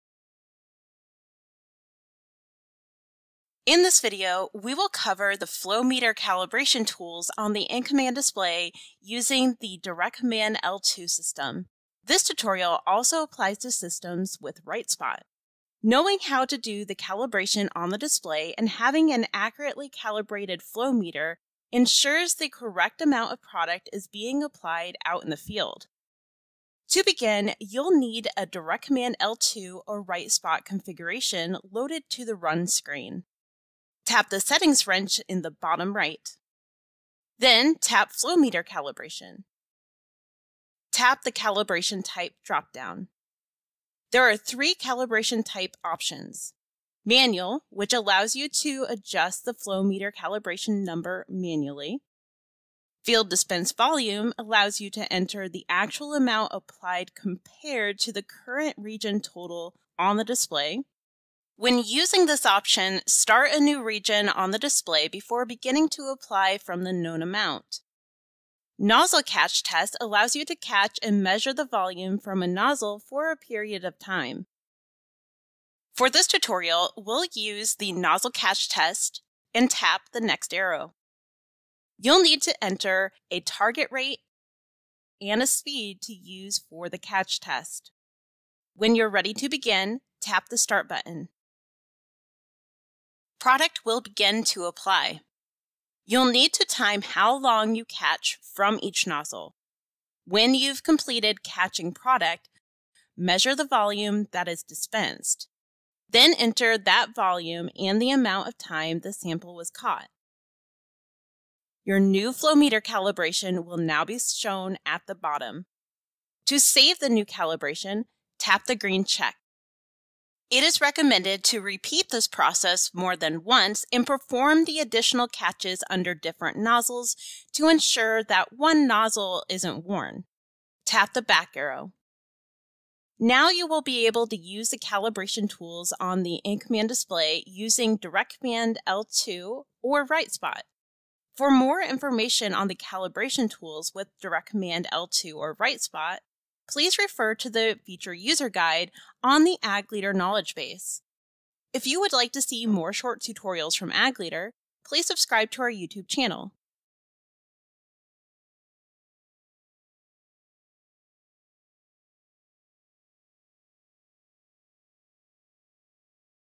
DirectCommand® L2 and RightSpot™ Flow Meter Calibrations
In this video, we will cover the flow meter calibration tools on the InCommand® display using the DirectCommand L2 system. This tutorial also applies to systems with RightSpot.